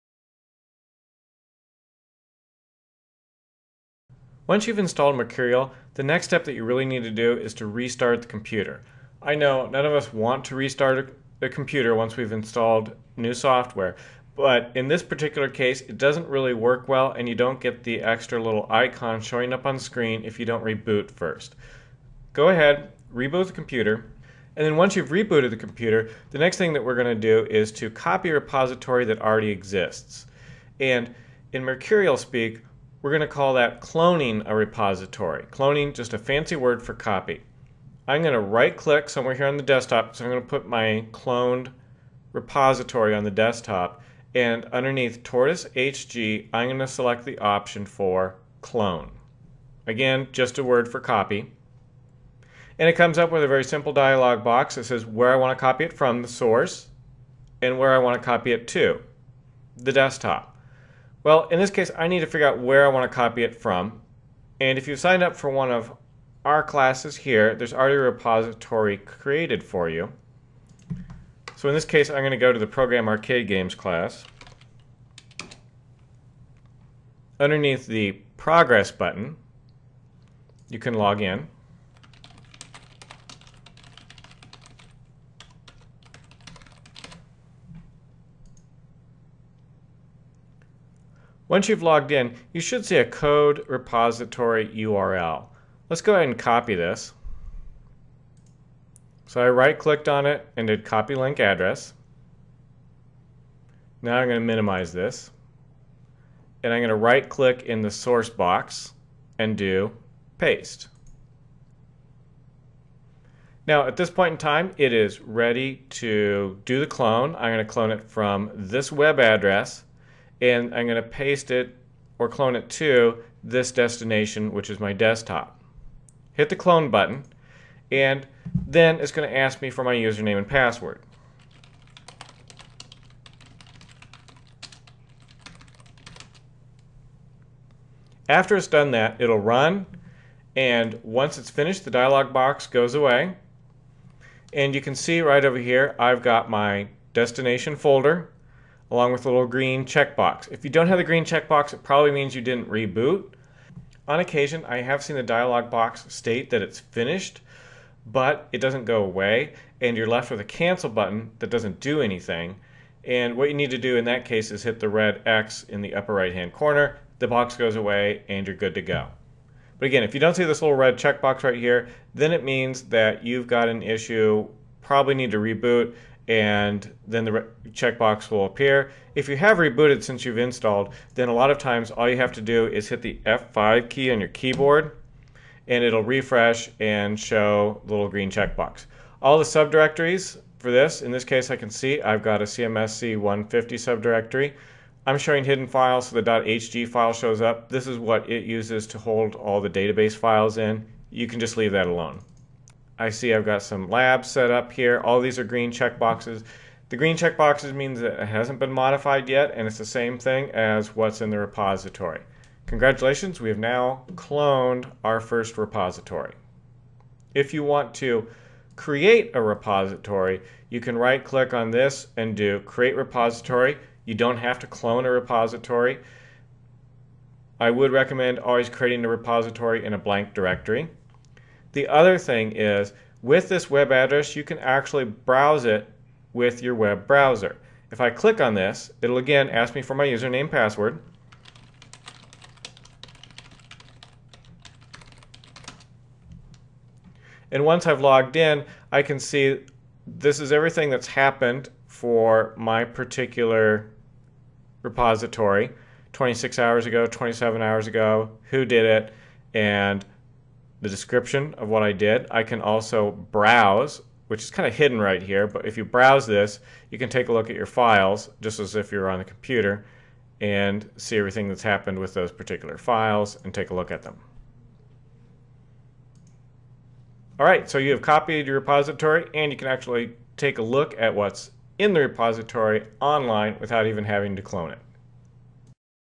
Appendix - Version Control: Clone code repository using TortoiseHg
Video tutorial on how to clone a Mercurial repository using TortoiseHg.